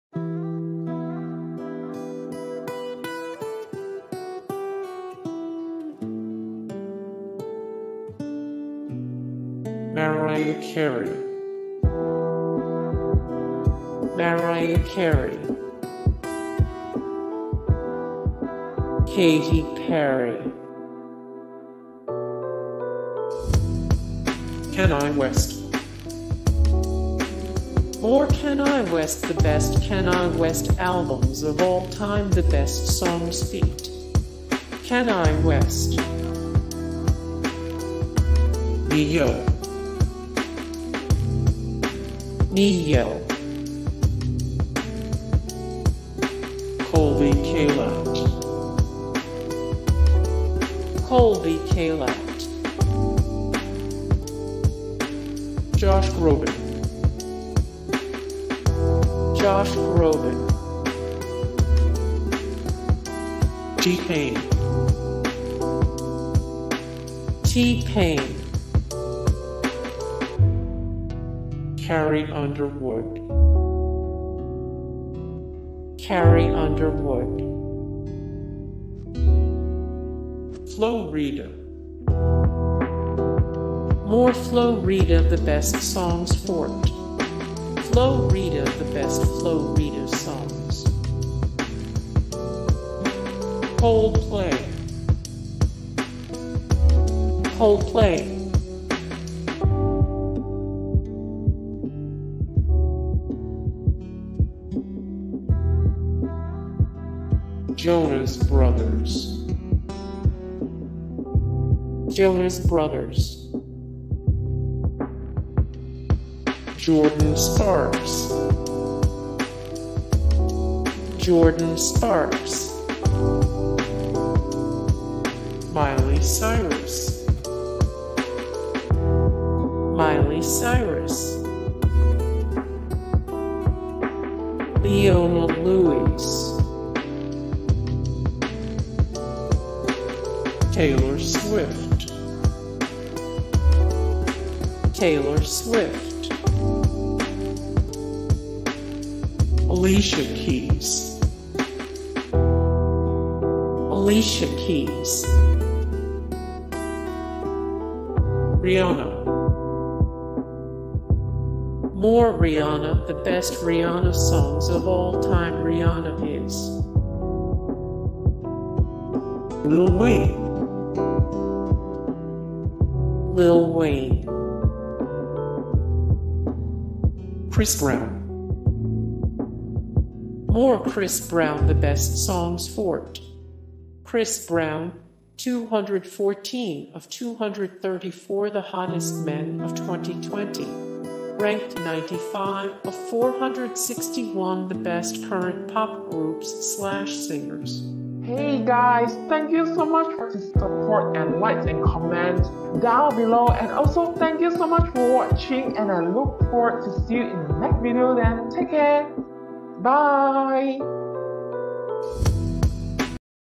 Billboard'S Top Musical Artists Of 2008 🧡
Billboard's Top Musical Artists of 2008. Some of these individuals are in the running to be the top recording artists of all time.For the full article go
0:00 - Intro
0:00:08 - Mariah Carey
0:00:18 - Katy Perry
0:00:25 - Kanye West
0:00:40 - Ne-Yo
0:00:49 - Colbie Caillat
0:00:59 - Josh Groban
0:01:08 - T-Pain
0:01:17 - Carrie Underwood
0:01:27 - Flo Rida
0:01:41 - Coldplay
0:01:50 - T.I.
0:01:57 - Jonas Brothers
0:02:07 - Jordin Sparks
0:02:17 - Miley Cyrus
0:02:27 - Leona Lewis
0:02:34 - Taylor Swift
0:02:44 - Alicia Keys
0:02:54 - Rihanna
0:03:06 - Lil Wayne
0:03:15 - Chris Brown